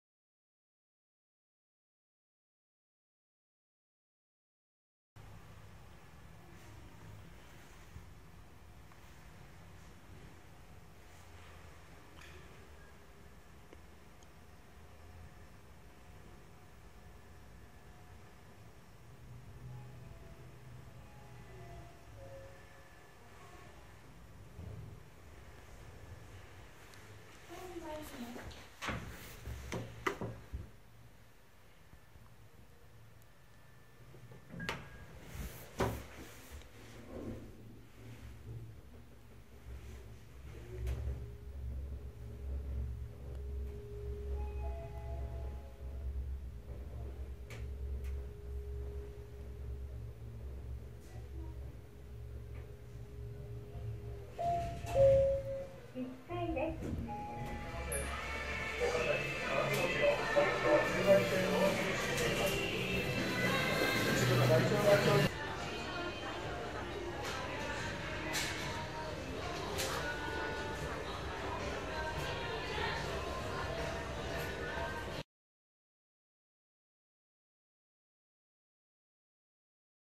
三菱エレベーター サミットストア王子店 Part1【FullHD】
サミットストア王子店のエレベーター(その１)です。

It is a Mitsubishi Electric Elevator of SUMMITO STORE OJI,Kita City,Tokyo Met in Japan.

ごく普通のグランディであるが、到着チャイム音が通常のとは音が高い物となっています。
カゴ外乗り場は1階と2階のみ、呼びボタンは特注の物で、インジケーターはドア上に設置されています。

メーカー：三菱
機種：グランディ
駆動方式：ロープ式
運転形式：群管理運転
設置台数：2基
音声案内：○
車椅子操作盤：○(左側のみ)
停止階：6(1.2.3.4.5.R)
定員：15名
積載：1000kg
設置年：1993年(平成5年)
撮影地：東京都北区王子　サミットストア王子店

Manufacturer:Mitsubishi Electric
Drive system:Rope type
Mode of operation:Group management operation
Number of installed:2
Announce:○
Wheelchair control panel:○(Left side only)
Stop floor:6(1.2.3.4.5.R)
Capacity:15
Loading:1000kg
Installation year:1993